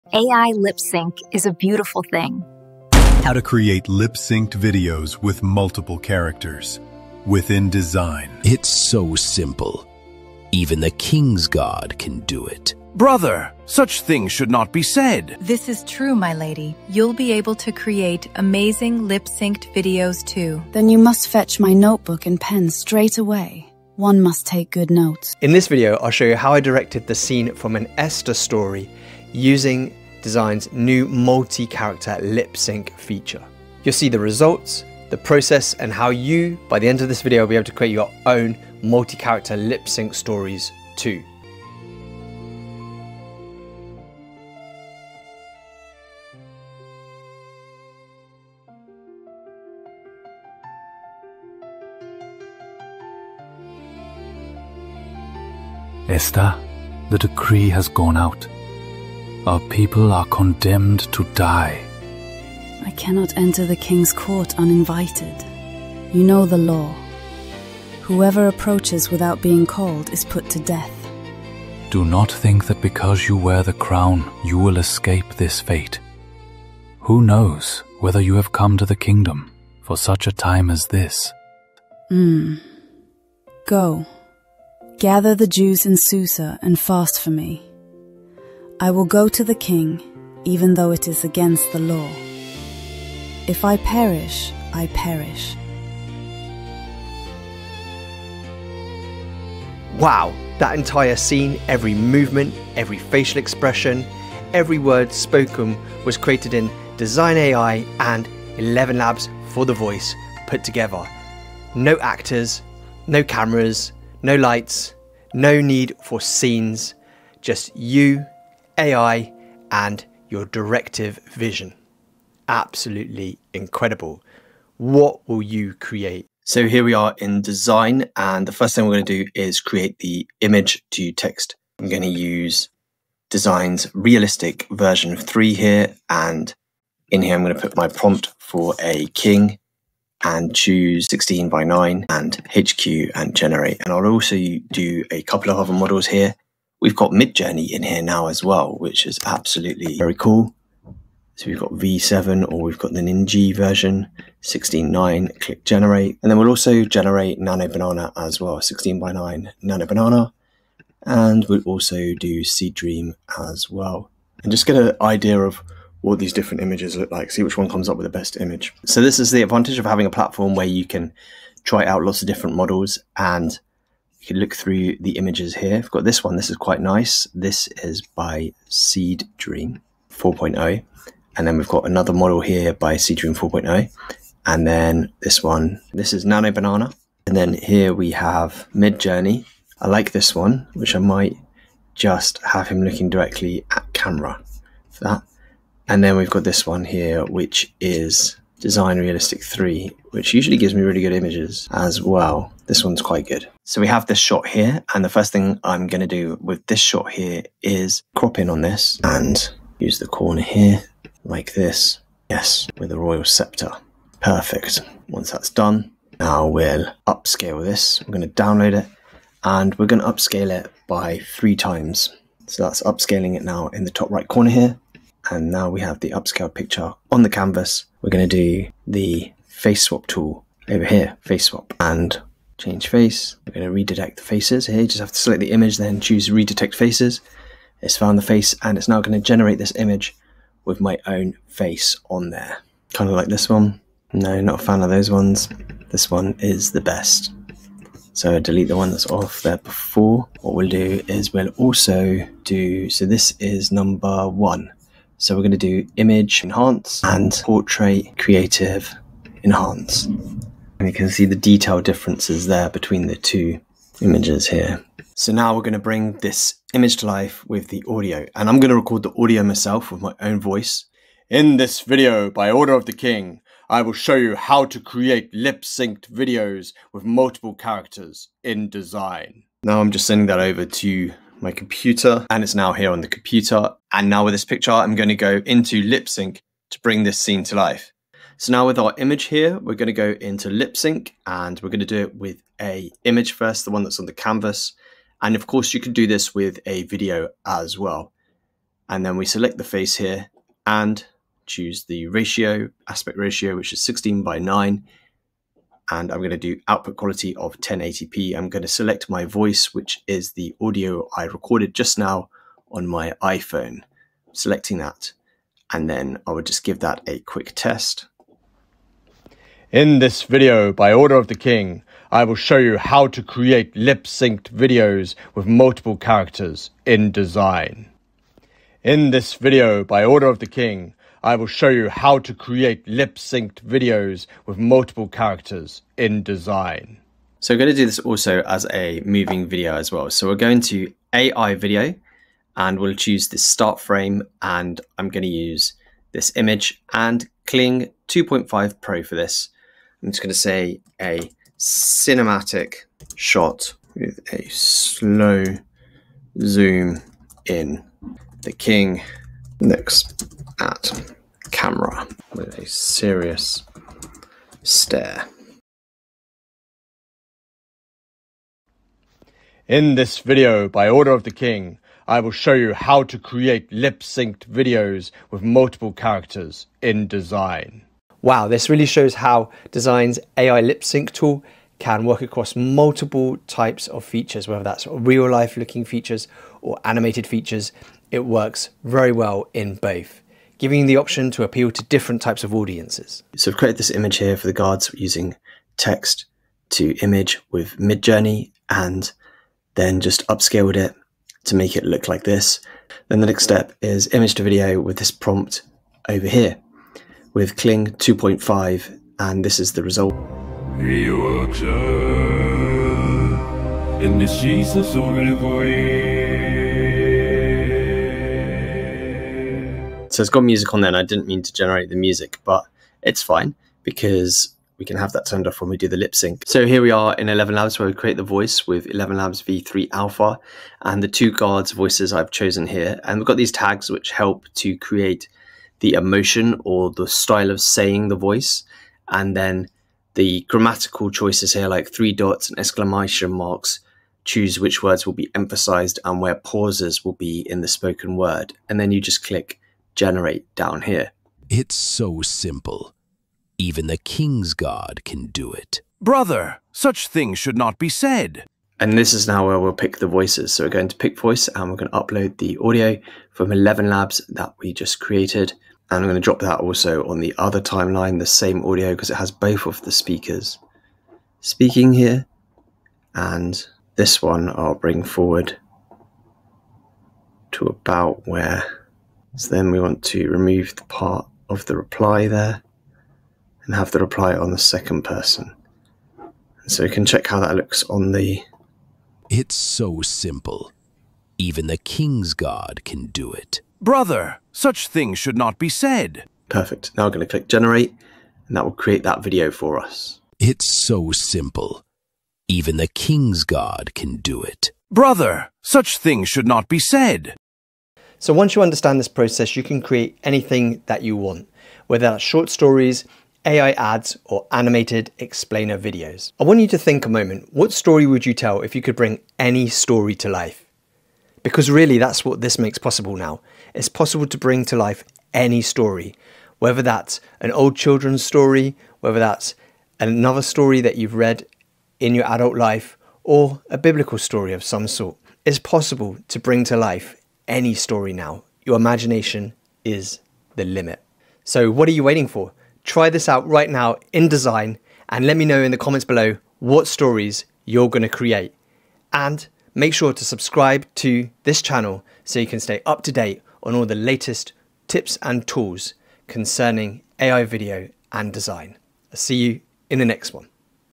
Dzine AI Takes Lip Sync to the Next Level
Learn how to master Dzine Lip Sync Pro and bring your characters to life in under 10 minutes.

Presented by:   @PureACT

In this creator-focused tutorial, we show you exactly how to turn a single image—or even multiple characters in the same scene—into fully lip-synced performances using Dzine’s powerful AI video tools.

You'll see how to:
 • Create realistic lip sync from images or video
 • Use Multi-Character Lip Sync for conversations
 • Generate characters using Text-to-Image inside Dzine
 • Add natural voices using ElevenLabs
 • Combine AI animation + lip sync for cinematic storytelling
 • Export polished videos for YouTube, TikTok, Reels, and more

Whether you're making short stories, ads, animations, or narrative scenes, this walkthrough gives you everything you need to create professional-level lip-synced content fast.

🕒 YouTube Chapters

0:00 AI Lip Sync Is a Beautiful Thing
0:03 How to Create Multi-Character Lip Sync in Dzine
0:08 Even the King's Guard Can Do It
0:13 “Such Things Should Not Be Said” — Dialogue Scene
0:25 What This Video Will Teach You
0:56 The Esther Scene Begins
1:02 “Our People Are Condemned to Die”
1:11 The Law of the King's Court
1:17 “For Such a Time as This”
1:23 Esther’s Decision: “If I Perish, I Perish”
1:40 Entire Scene Created With Dzine AI + ElevenLabs
2:04 What Will You Create?
2:07 Step 1 — Creating the Characters (Text-to-Image)
2:49 Testing Multiple Models (MJ, SeedDream, NanoBanana)
3:24 Choosing and Cropping the Final Image
3:35 Upscaling for Maximum Quality
3:48 Face Swap: Inserting Yourself Into the Scene
4:12 Enhancing the Portrait
4:26 Step 2 — Recording Your Voiceover
4:48 Step 3 — Lip Syncing the Image
5:28 Previewing the Lip Sync Result
5:38 Creating a Moving Version With Image-to-Video
6:19 Realistic vs Animated Lip Sync Examples
6:33 Creating the Guard Characters
7:00 Guard Animation Results
7:06 Building Voices in ElevenLabs V3
7:31 Assigning Voices to Each Character
8:13 Multi-Character Lip Sync Preview
8:17 Final Multi-Character Generation
8:28 What You Can Create With Dzine Lip Sync
8:36 Any Story Can Be Brought to Life
9:03 Try It Yourself — And Share Your Story
9:10 Subscribe for More AI Video Tutorials